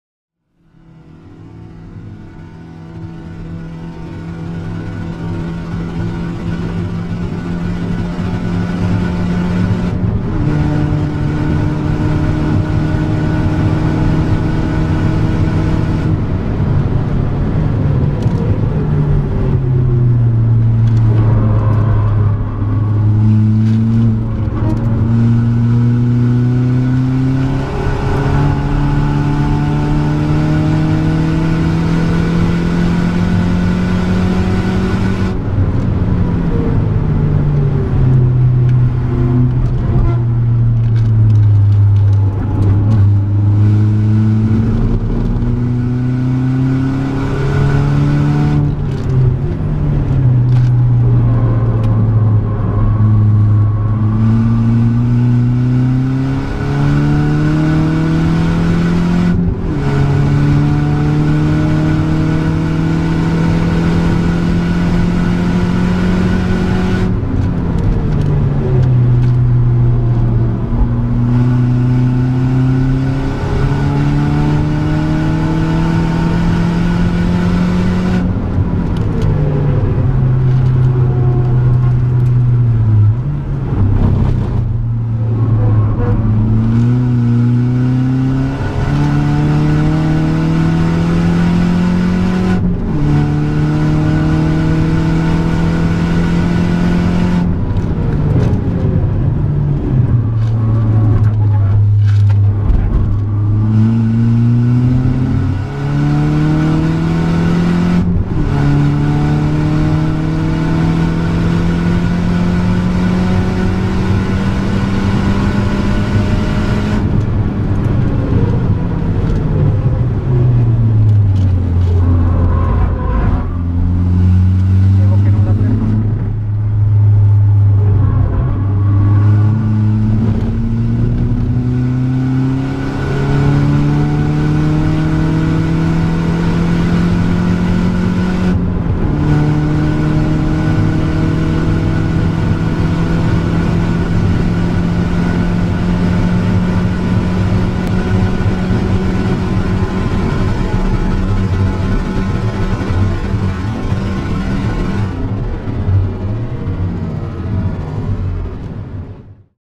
Imola Polo GTI 6c 30.04.2019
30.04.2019
Best Lap  2:16.10
Optimal 2:14.90